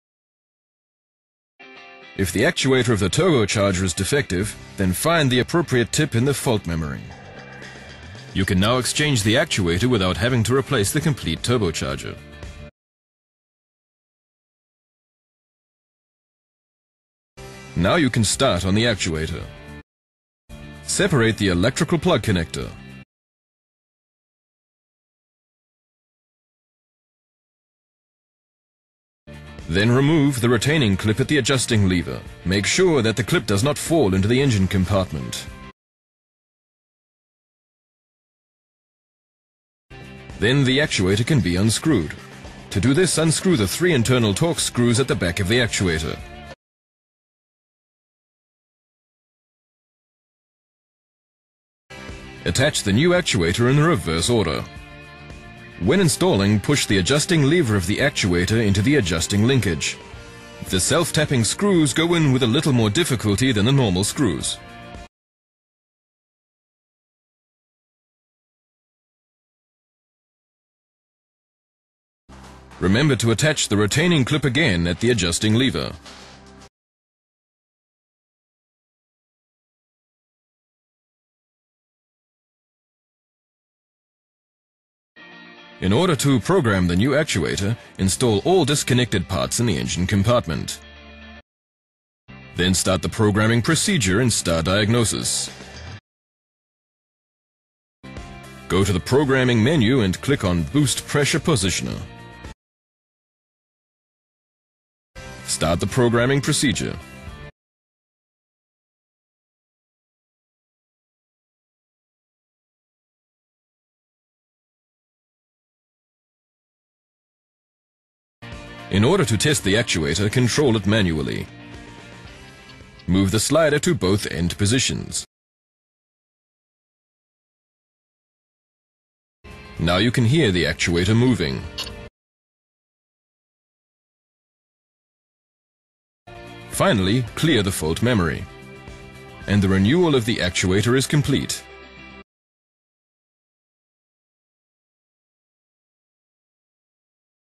Mercedes-Benz - How to replace the actuator motor on turbocharger OM651 red | W204, W212, C207, X204
If the actuator motor of the turbocharger  is defective, there will be a message  in the fault memory.
The actuator motor can be replaced without replacing the entire turbocharger.
The actuator motor is now easily accessible.
Disconnect the electrical connector.
Remove the retaining clip on the actuator arm.
Make sure that the retaining clip does not fall into the engine compartment.
In order to unscrew the motor,  remove the three internal Torx bolts  at the rear of the motor.
Attach the new actuator motor in the reverse order.
When inserting, slide the actuator lever of the motor into the actuating linkage.
The self-tapping screws are slightly more difficult to screw in than normal screws.
Reattach the retaining clip to the actuator lever.
Before teaching in the engine, attach all removed parts in the engine compartment.
Start the teach-in process with Star Diagnosis.
Call up the teach-in menu and select the boost pressure regulator.
Start the teach-in process.
Manually actuate the actuator motor to test it.
To do so, move the slider to both end positions.
You can hear the movements  of the actuator motor.
After erasing the fault memory, replacement of the actuator motor is complete.